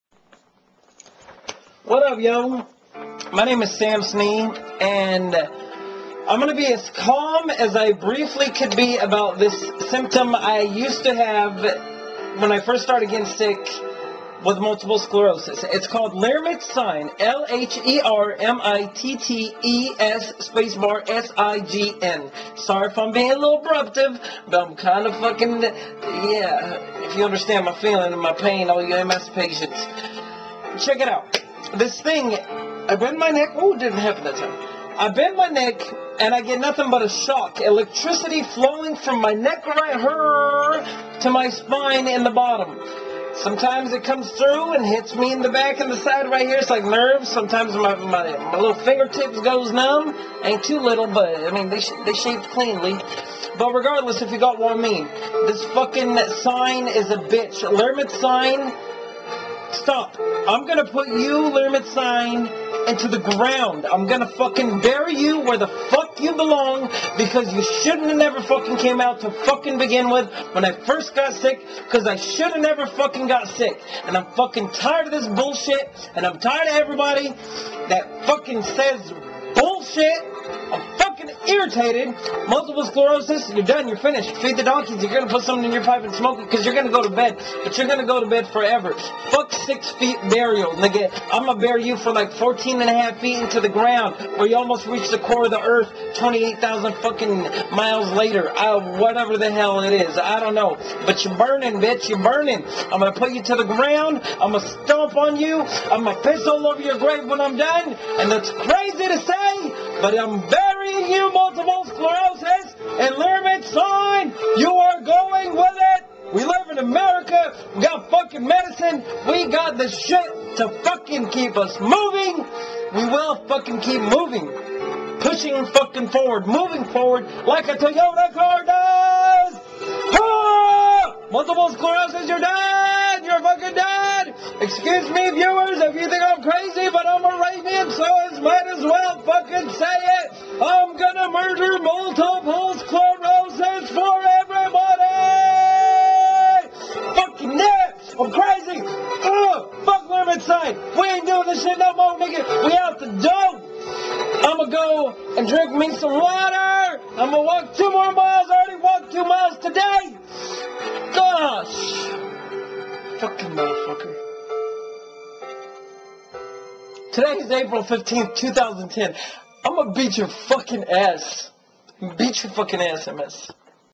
MULTIPLE SCLEROSIS AND LHERMITTES SIGN
A FRUSTRATING ELECTIRC FLOW DOWN THE SPINE But a shock every tyme. . . .

(ELECTRICITY)